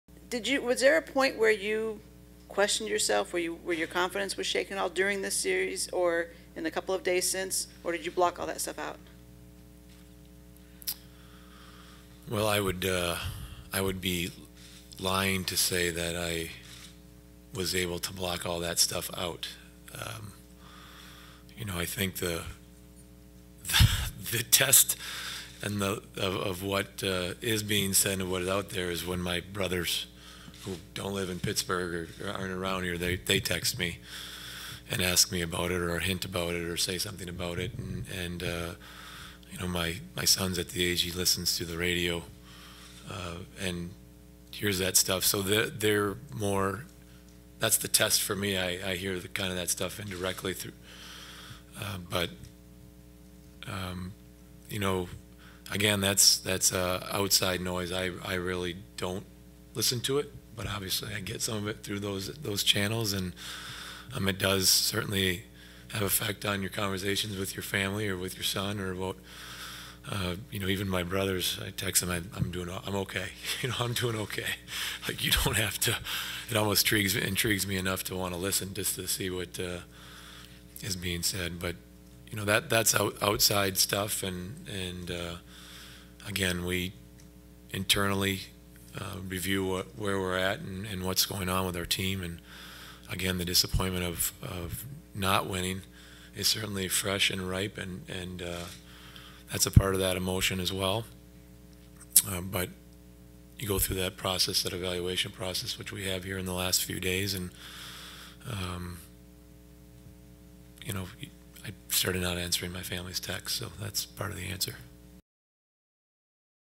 Bylsma talks media & public opinion
Penguins coach Dan Bylsma discusses the media and public opinion.